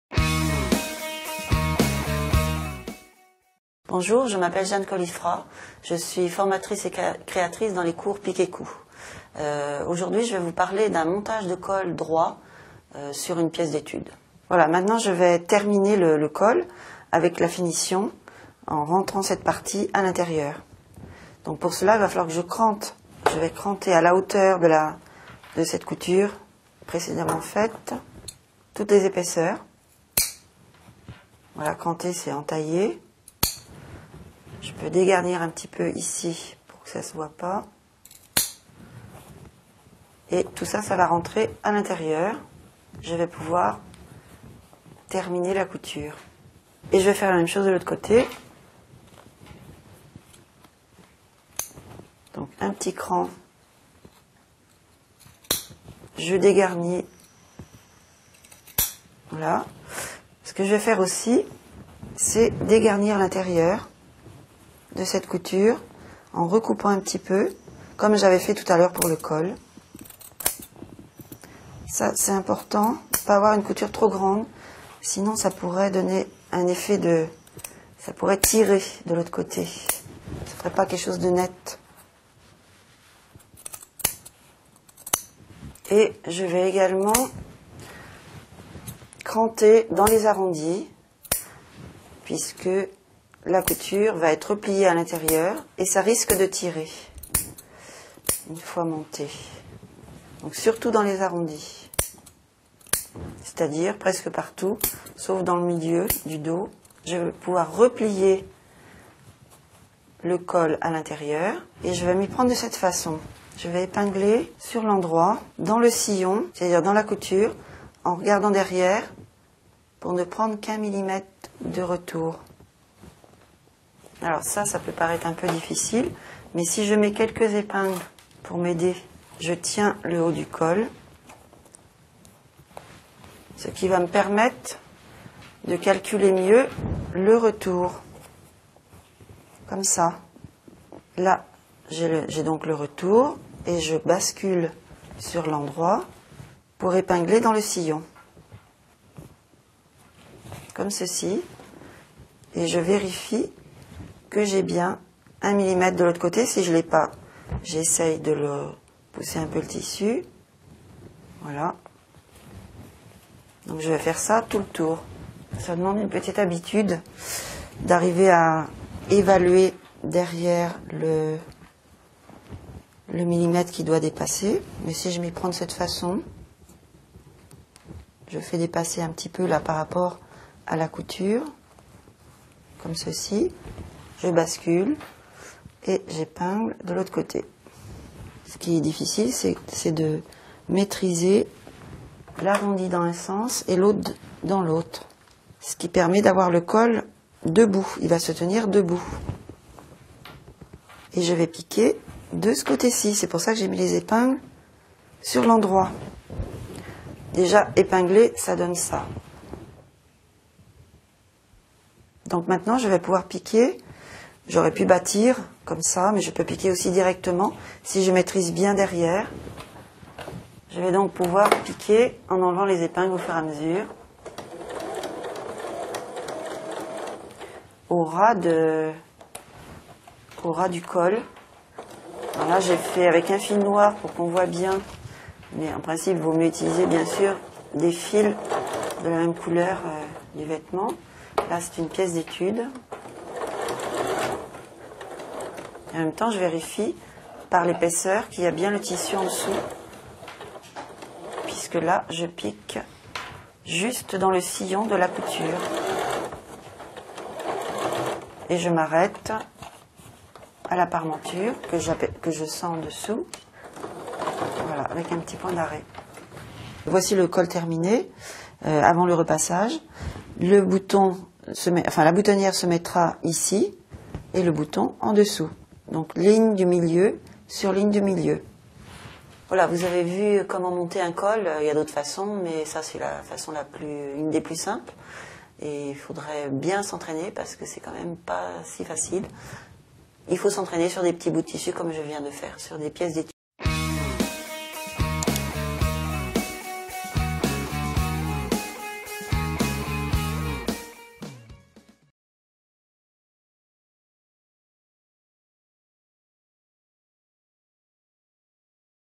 Comment coudre un col droit sur son encolure
Comment coudre un col droit sur son encolure ? Comment monter un col droit ? Regardez la technique est très simple à réaliser.

Jean Kolifrat, créatrice et formatrice dans les cours « Piques et couds » (pique-coud.com), vous explique comment faire.